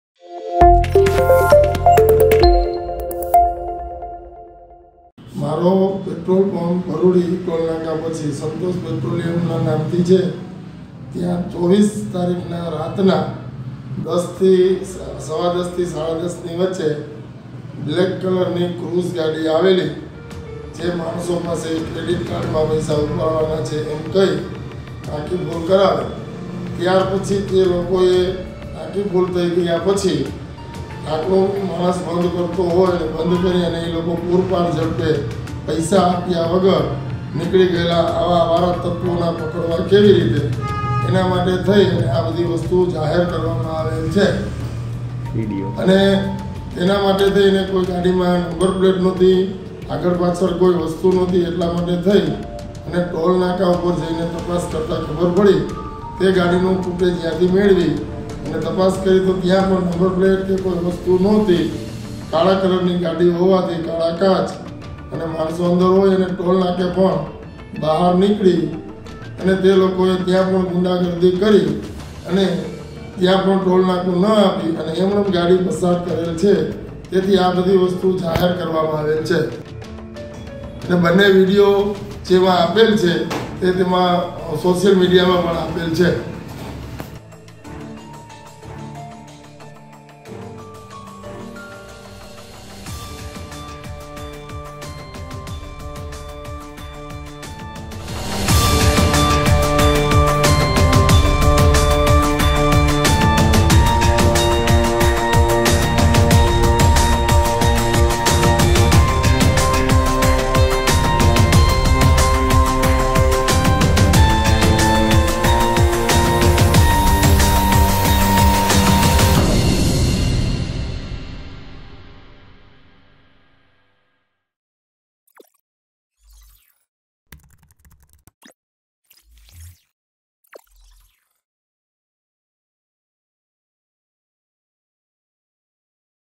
લુખ્ખાઓનો આતંક, પેટ્રોલ પુરાવી કર હકાલી મૂકી, CCTV માં કેદ
chandninews rajkot chandaninews | લુખ્ખાઓનો આતંક, પેટ્રોલ પુરાવી કર હકાલી મૂકી, CCTV માં કેદ